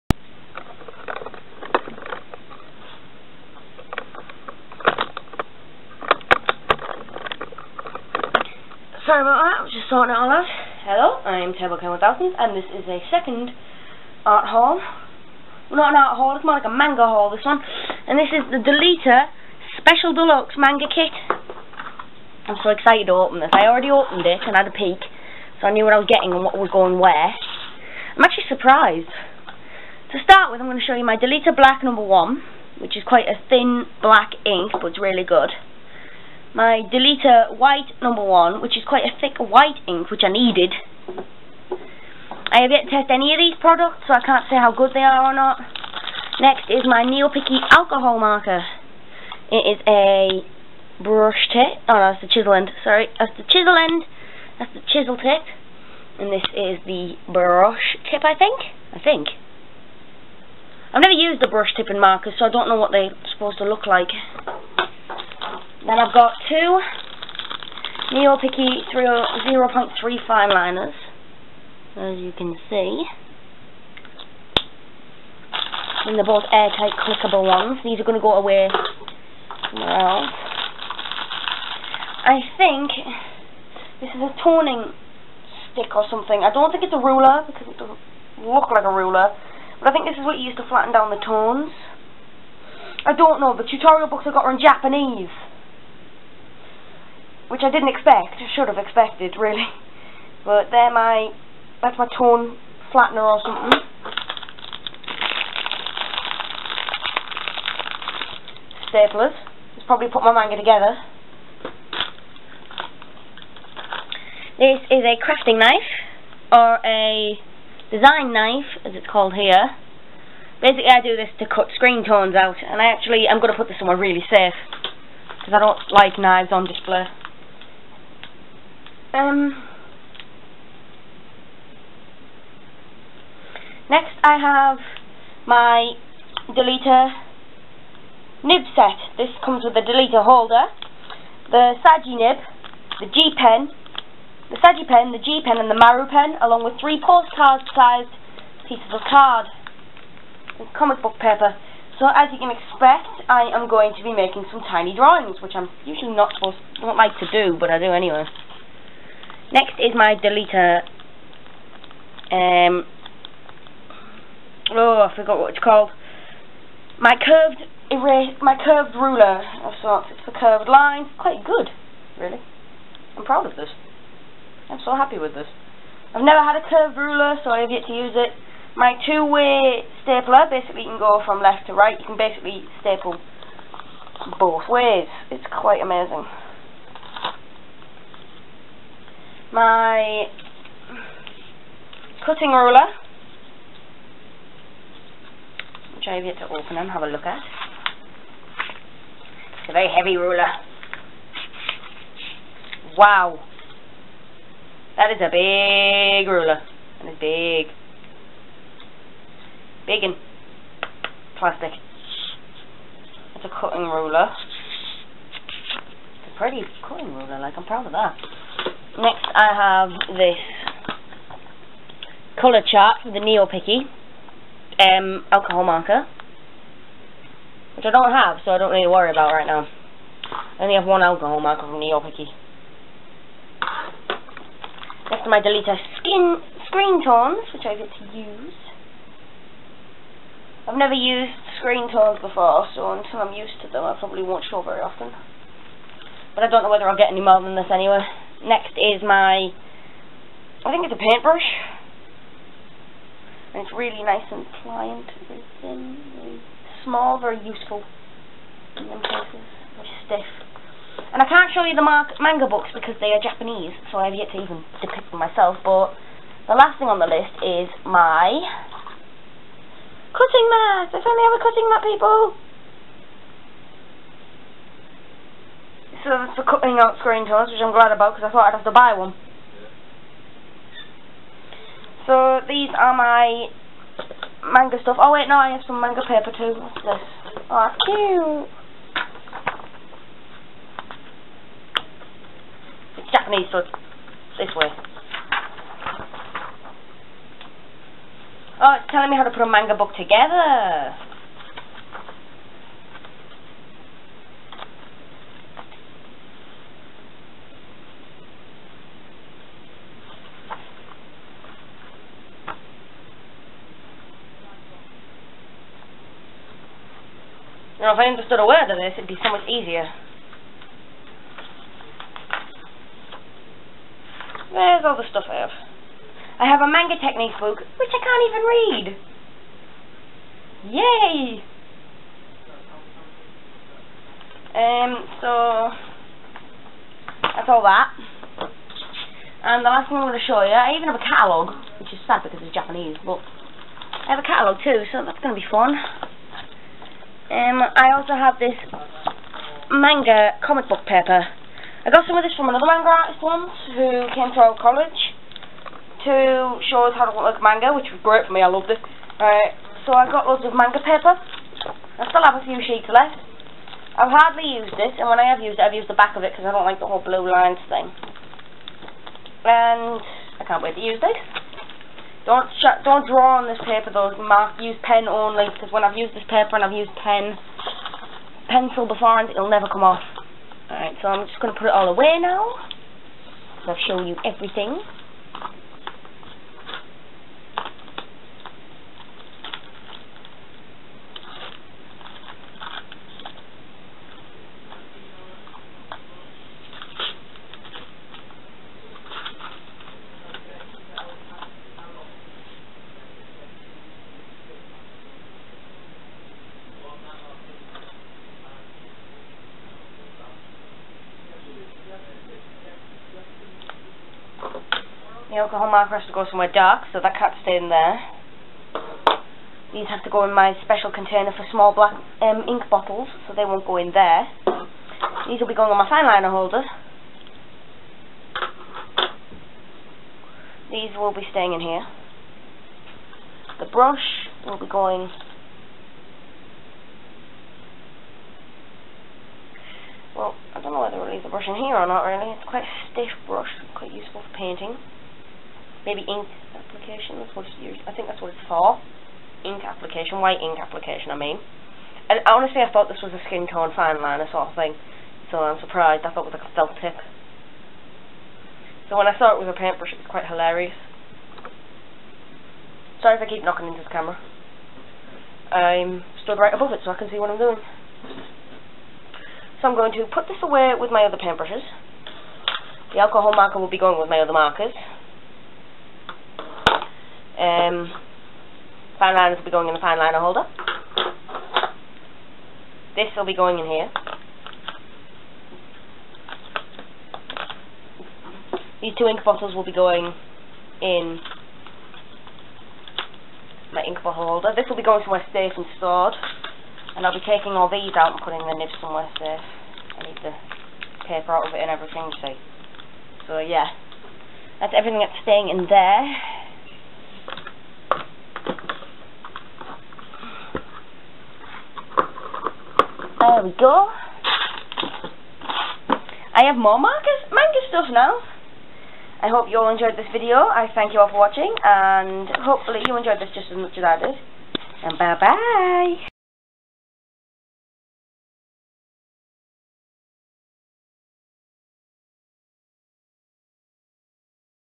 Manga Haul: Deleter SPDX Kit
Please Rate, Comment and Subsccribe!

I am so excited to open this right now, it's so cool, there are so many thing in this set it's unreal. Also, I was wrong, there aren't two 03 fine liners, one is actualy 005 and I didn't notice.